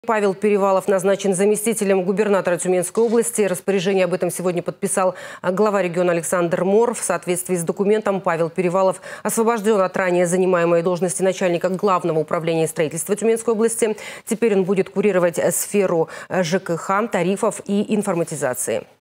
Павел Перевалов назначен заместителем губернатора Тюменской области
Павел Перевалов назначен заместителем губернатора Тюменской области. Распоряжение об этом 26 сентября подписал глава региона Александр Моор.